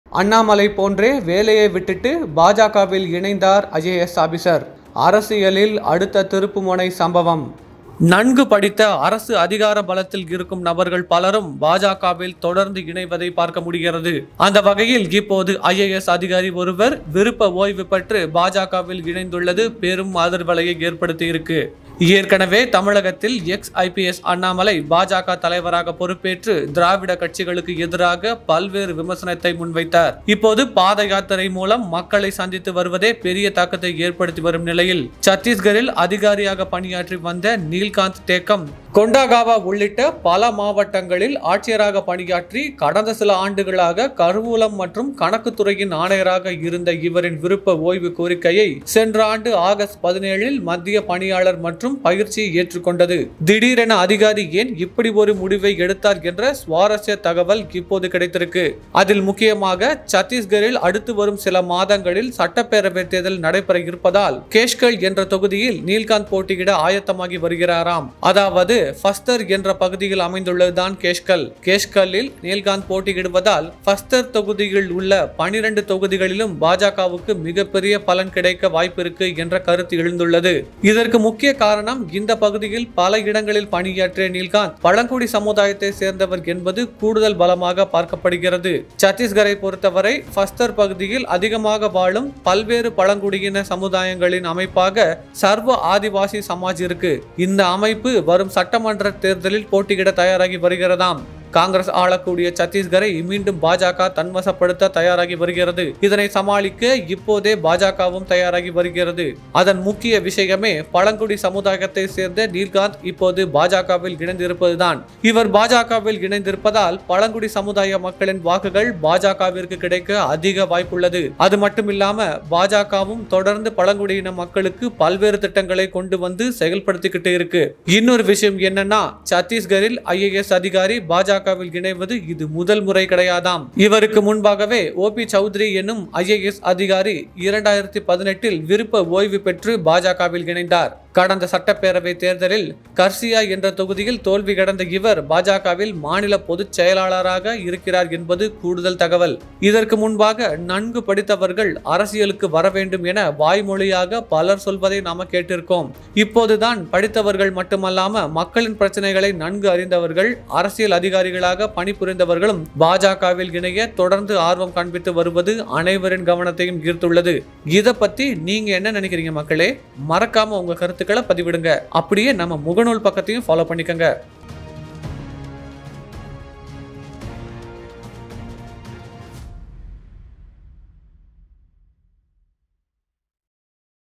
அண்ணாமலை போன்றே பாஜக-வில் இணைந்தார் ஐஏஎஸ் ஆபிசர்.. அரசியலில் அடுத்த திருப்புமுனை சம்பவம்..!
அண்ணாமலை போன்றே வேலையை விட்டுட்டு பாஜக - வில் இணைந்தார் ஐஏஎஸ் ஆபிசர்... அரசியலில் அடுத்த திருப்புமுனை சம்பவம்..!

எதார்த்த அரசியல்- சினிமா கள நிலவரம் மற்றும் பயனுள்ள தகவல்களை தெரிந்துகொள்ள TM Infotainment சேனலை பின்பற்றிக்கொள்ளவும்